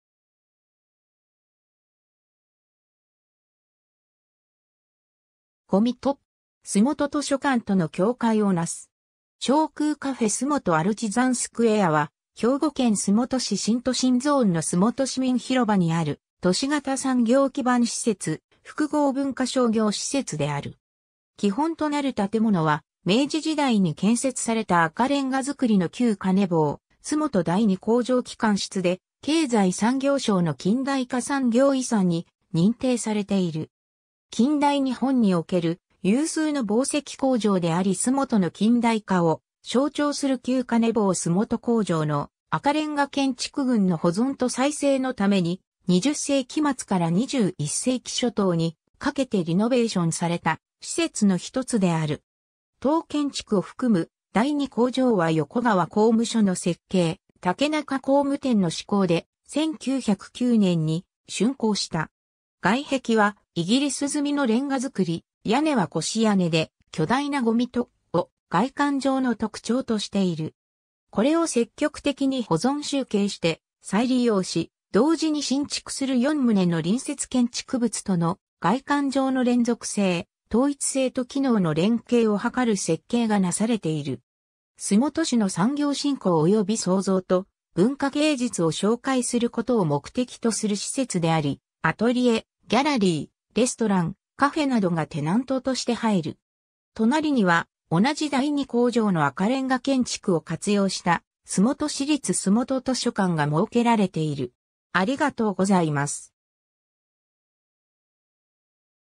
洲本アルチザンスクエア
塵突。
洲本図書館との境界を成す。
 小空カフェ 洲本アルチザンスクエア（すもとアルチザンスクエア）は兵庫県洲本市新都心ゾーンの洲本市民広場にある都市型産業基盤施設、複合文化商業施設である。
 基本となる建物は明治時代に建設された赤レンガ造の旧鐘紡洲本第二工場汽缶室で、経済産業省の近代化産業遺産に認定されている。
 近代日本における有数の紡績工場であり洲本の近代化を象徴する旧鐘紡洲本工場の赤レンガ建築群の保存と再生のために、20世紀末から21世紀初頭にかけてリノベーションされた施設の一つである。
 当建築を含む第二工場は横河工務所の設計、竹中工務店の施工で、1909年（明治42年）に竣工した。
 外壁はイギリス積みの煉瓦造、屋根は越屋根で、巨大な塵突を外観上の特徴としている。
 これを積極的に保存修景して再利用し、同時に新築する4棟の隣接建築物との外観上の連続性・統一性と機能の連携を図る設計がなされている。
洲本市の産業振興および創造と文化芸術を紹介することを目的とする施設であり、アトリエ、ギャラリー、レストラン、カフェなどがテナントとして入る。
隣には同じ第二工場の赤レンガ建築を活用した洲本市立洲本図書館が設けられている。